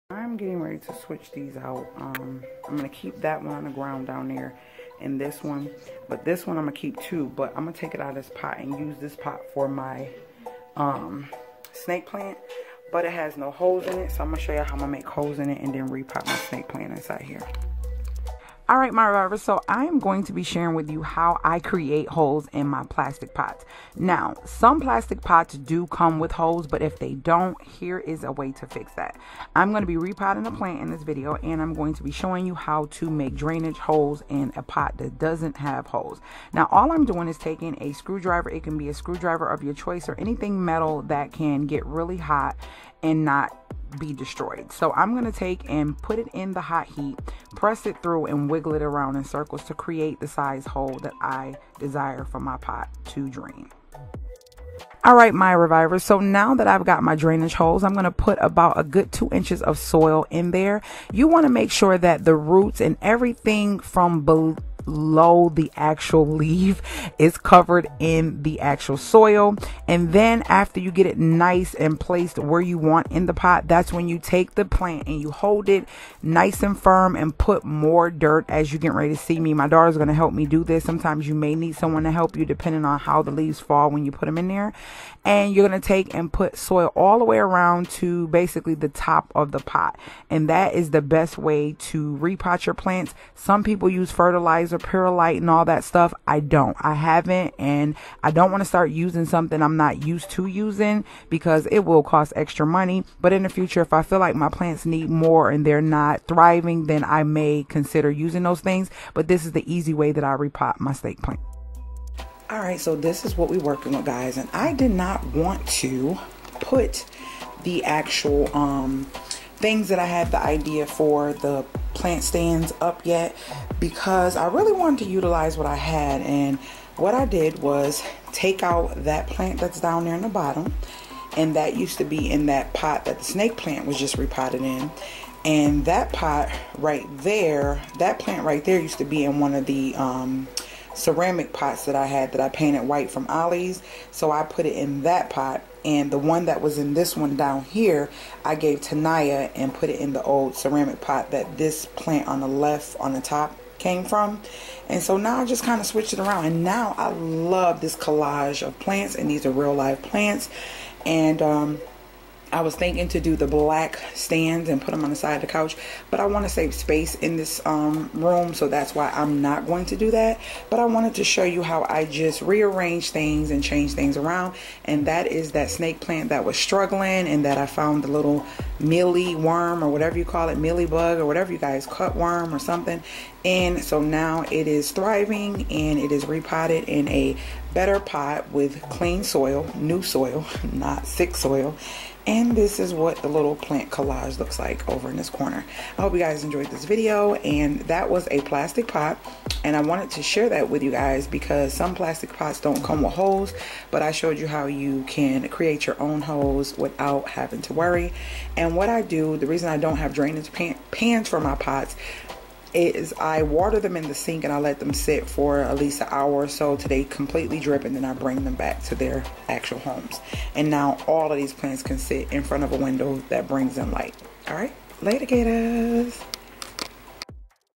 How To Repot A Snake Plant And Create Drainage Holes In A Plastic Planter
How to repot a snake plant

The CPR Vibe  8pm every Sunday

BUFFALO NY,14215
$CPRvibe

Please go support both of my prince's channel's